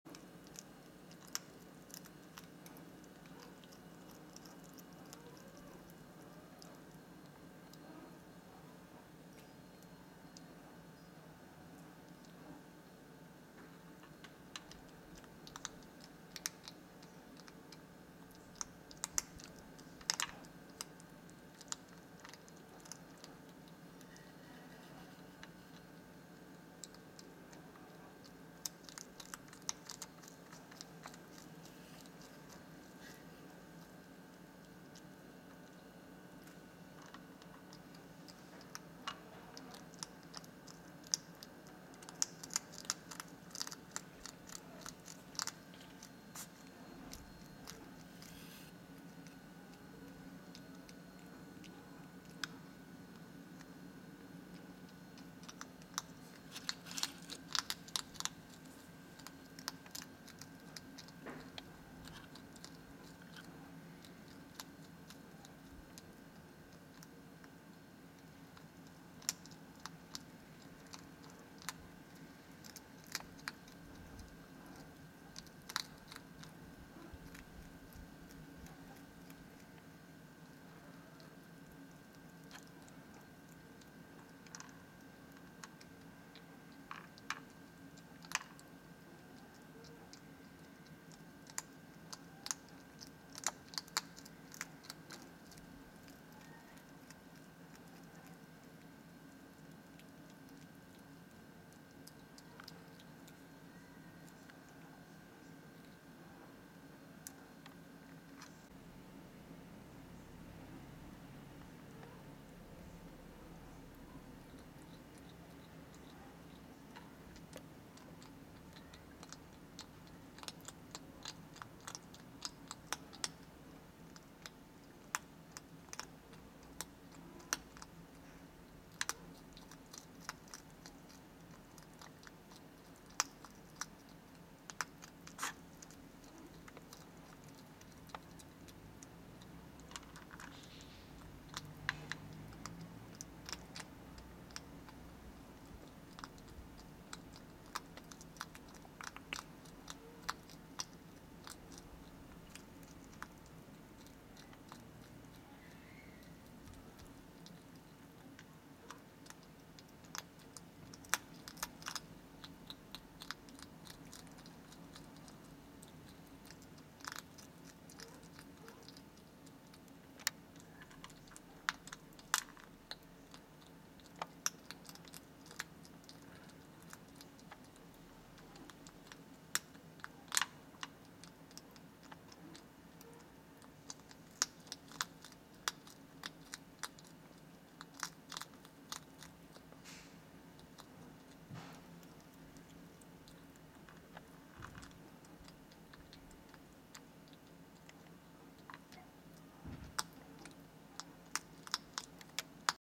Mon Hérisson casse la croûte (ici les croquettes du chat) leforest wadeshadow
ATTENTION JAMAIS DE LAIT POUR UN HÉRISSON CA LES TUES